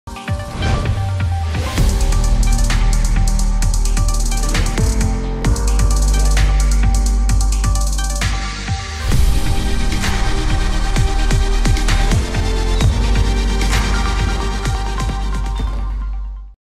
GLOBAL TITANS - Dubai NFT
The world's first NFT enabled livestream sports event! Explosive card of fighters including Floyd Mayweather v Don Moore, Anderson SIlva v Bruno Machado, Badou Jack v Hany Atiyo.